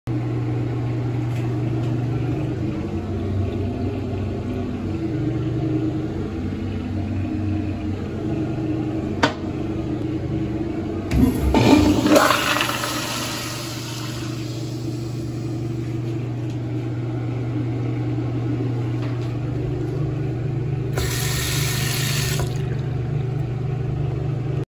AS madera at resource one
Toilet rating: E+

These toilets are junk  and these clog easily on weak pressure.